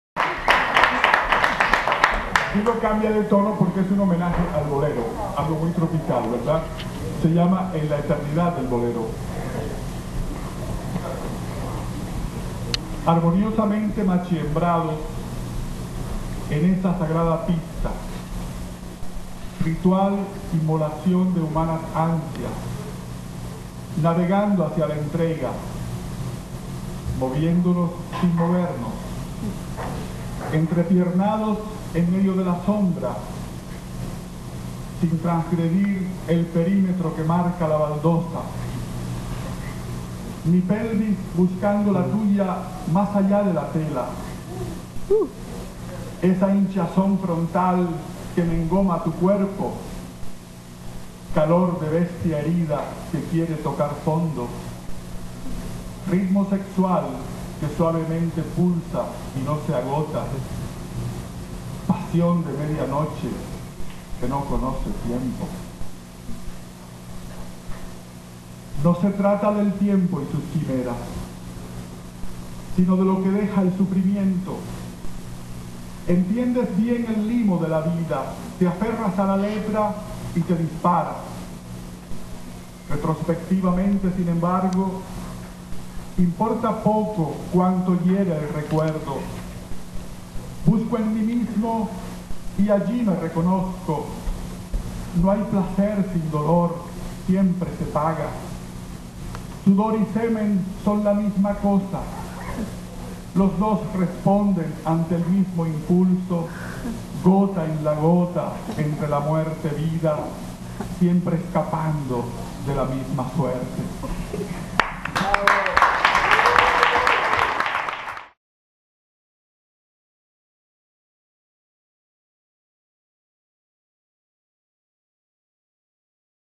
EN LA ETERNIDAD DEL BOLERO
"En la eternidad del bolero" (poema) de Teobaldo Noriega.

noche de poesia en el S.A.C.